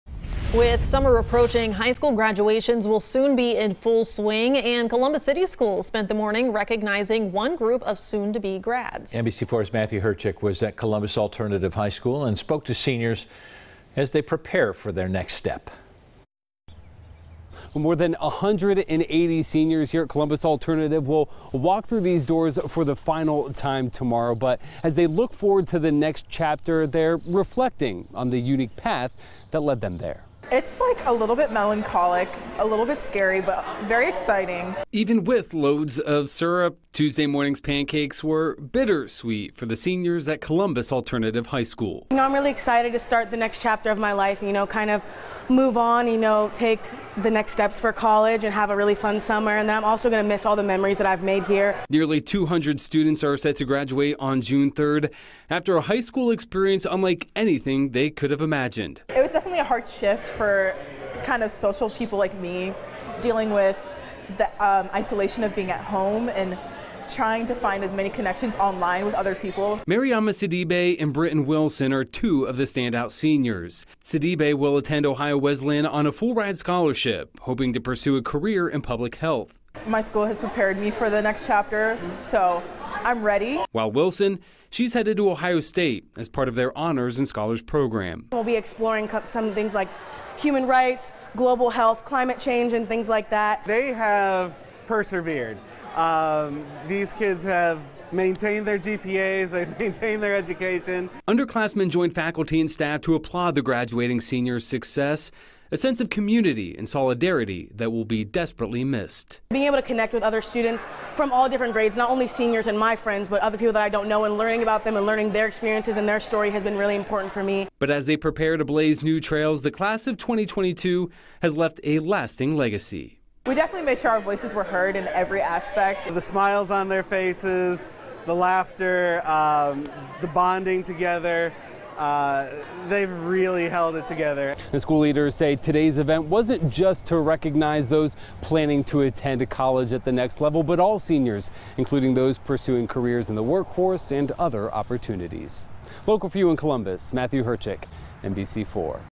Graduating seniors recognized
COLUMBUS, Ohio (WCMH) – With summer approaching, high school graduations will soon be in full swing.